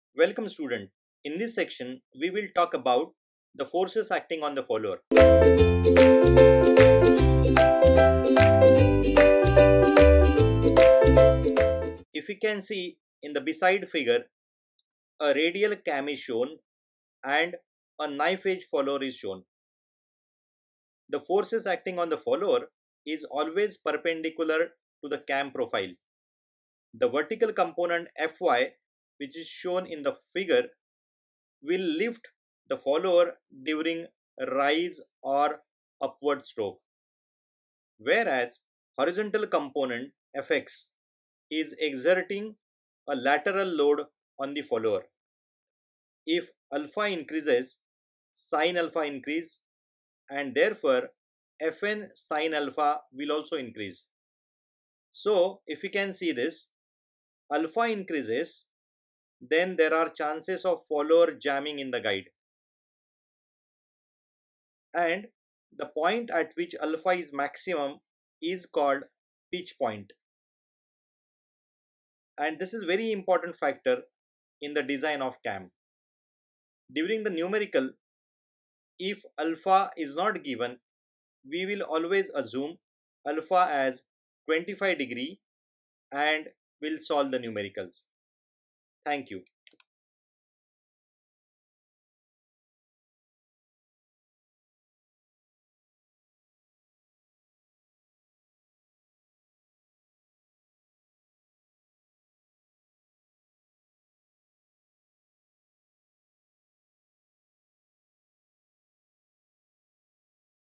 Force Acting on Cam - Design of Cam and Followers - Machine Design - 2
Subject - Machine Design - 2

Video Name - Force Acting on Cam

Chapter - Design of Cam and Followers

Faculty - Prof. Mangesh Mohan

Happy Learning!